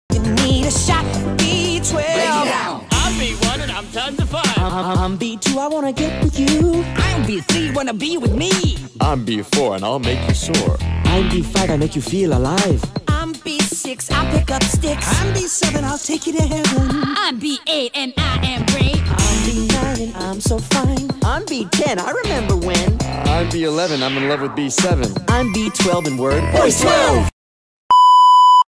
Boys 12 meme but it’s my rainbow friends ocs
The new ones:

Indigo is the second leader of the rainbow friends. Other than red, indigo is very nice and caring and help anybody in need￼￼￼￼.

Ginger￼ is oranges ￼cousin (that’s why they almost look alike) he has the same personality as orange, but instead of being hungry he’s always ￼ sleeping.

Brown is a Chinese person so she’s so obsessed with anime. And her face is somehow unknown. Always ￼ peaceful.

￼ mustard is the only one who is actually an real animal ￼￼￼￼￼￼￼(he’s a armadillo). ￼ he’s everyone’s good friend except for orange and red.

The others are in the community tab. While electric blue is in one of my videos.￼￼￼￼￼